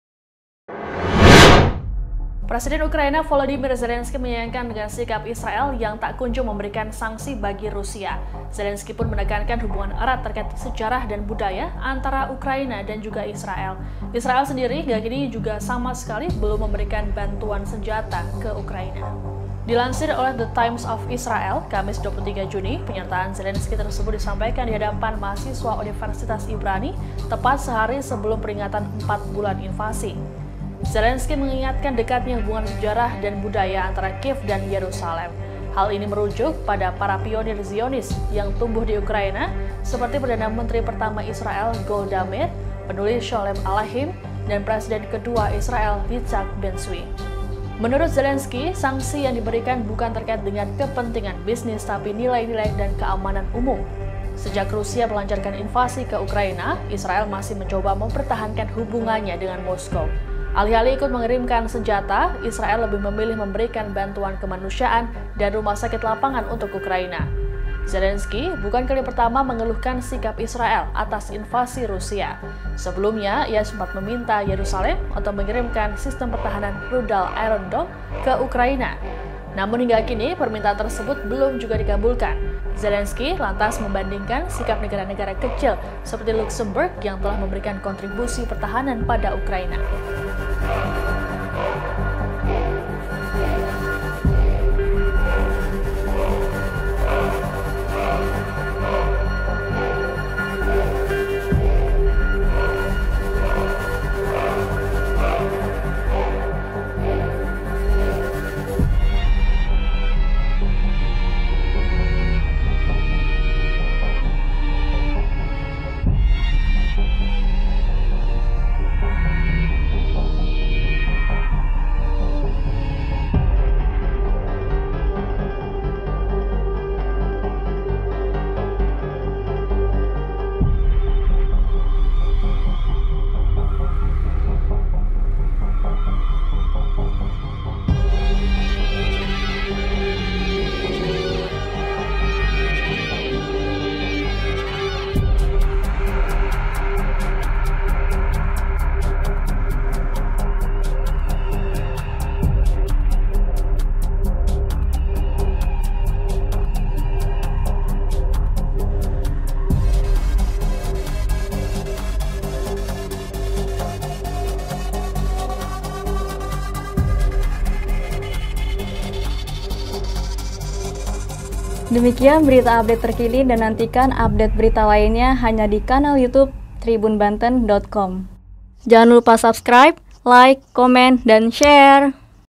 Zelensky Menyayangkan Sikap Israel yang Tak Memberikan Sanksi Bagi Rusia
TRIBUNBANTEN -  Presiden Ukraina Volodymyr Zelensky menyayangkan sikap Israel yang tak memberikan sanksi bagi Rusia.

Zelensky pun menekankan hubungan erat terkait sejarah dan budaya antara Ukraina dengan Israel.

Israel sendiri hingga kini juga sama sekali belum memberikan bantuan senjata ke Ukraina.